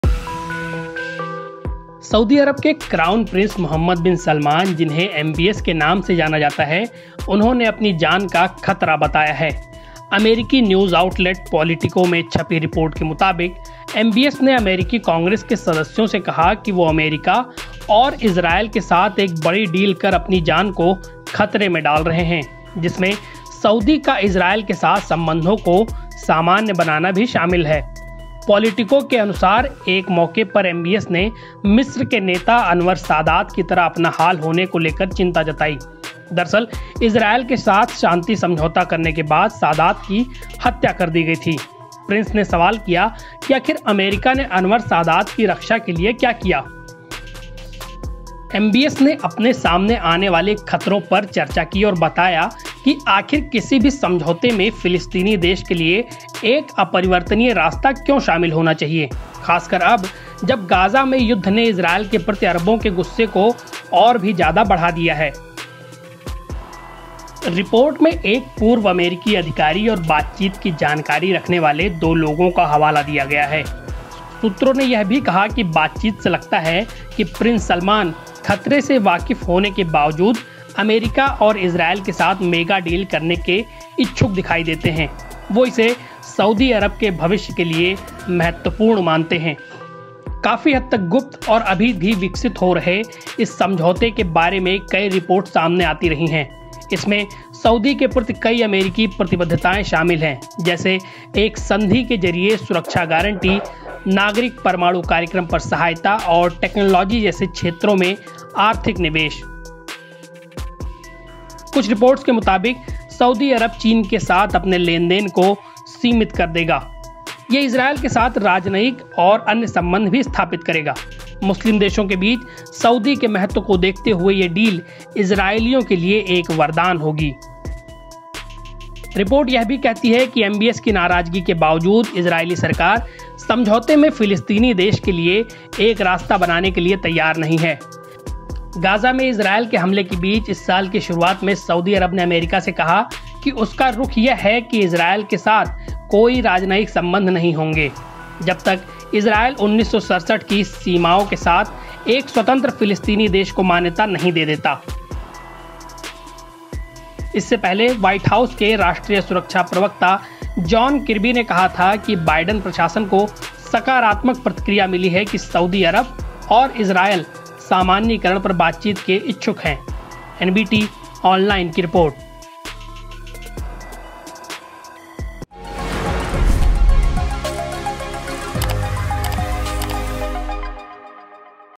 Saudi Crown Prince MBS की जान खतरे में खुद बताया कब होगी हत्या की कोशिश | Israel | NBT
रियाद: सऊदी अरब के क्राउन प्रिंस मोहम्मद बिन सलमान ने अपनी जान को खतरे में बताया है। एक रिपोर्ट में कहा गया है कि एमबीएस ने अमेरिकी अधिकारियों को बताया कि इजरायल से अगर संबंध सामान्य किए जाते हैं तो उनकी हत्या की साजिश रची जा सकती है। उन्होंने फिलिस्तीन बनाने की भी मांग की है।